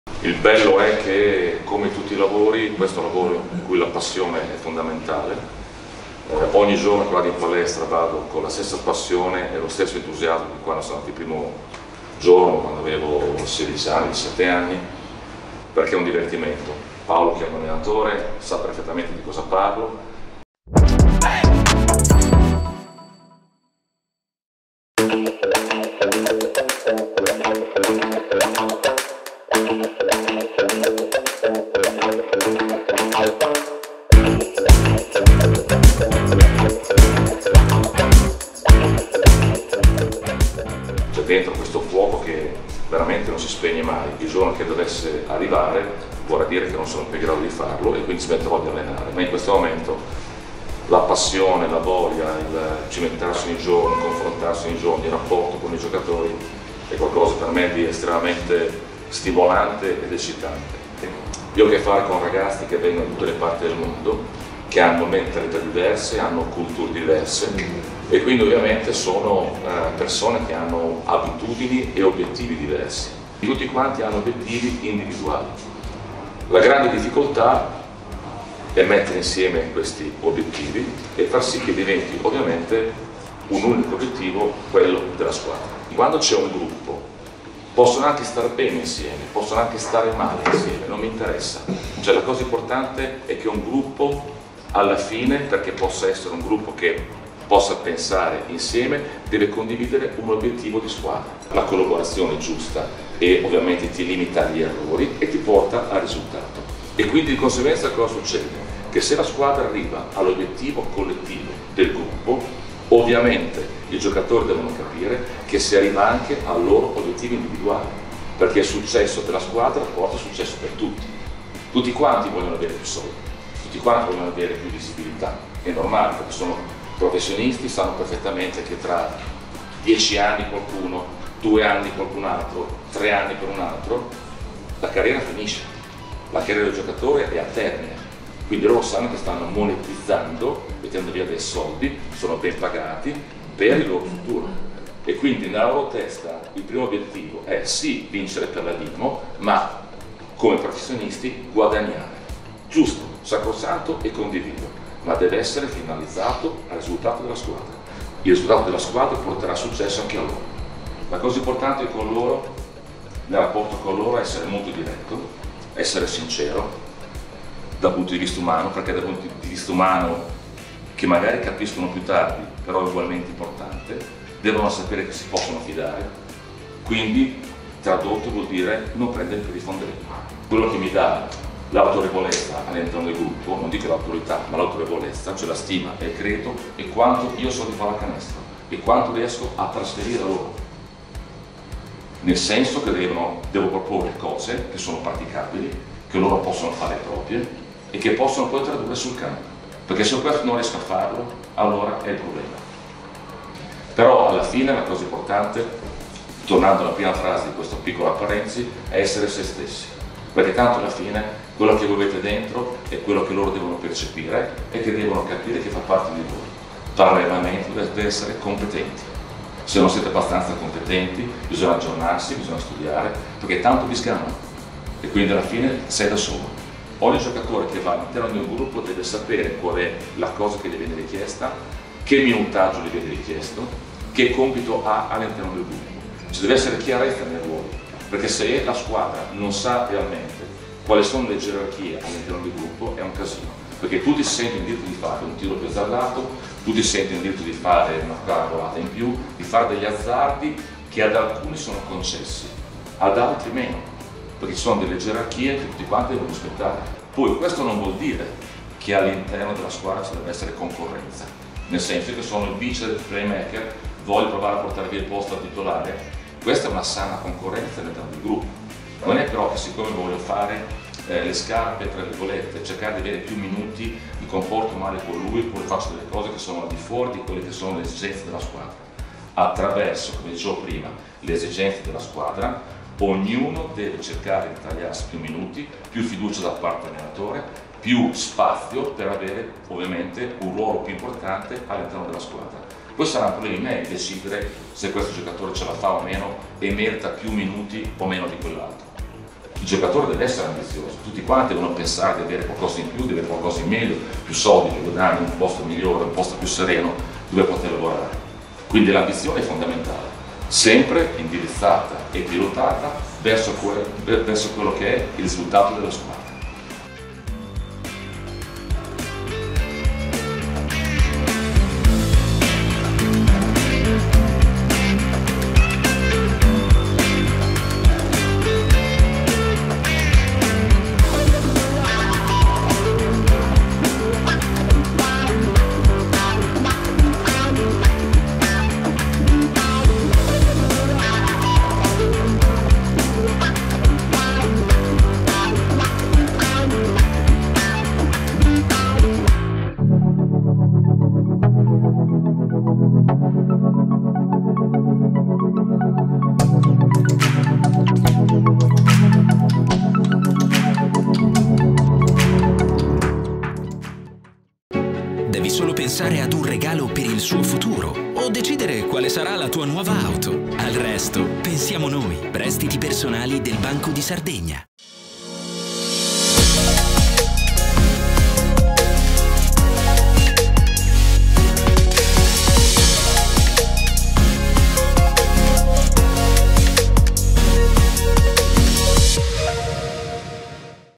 LA "LECTIO" DI COACH PIERO BUCCHI
Nei giorni scorsi, la Dinamo Banco di Sardegna ha incontrato la classe del percorso di general management della PMI Academy. Dopo il saluto del capitano e gli interventi biancoblu del direttore generale Viola Frongia, del direttore marketing Luigi Peruzzu, del Comunication Director Paolo Citrini e del Brand awareness director Valentina Sanna, è salito in cattedra coach Piero Bucchi, con una bellissima "lectio" sul gruppo. Dinamo Tv ha realizzato la clip video sui passaggi salienti dell'allenatore del Banco.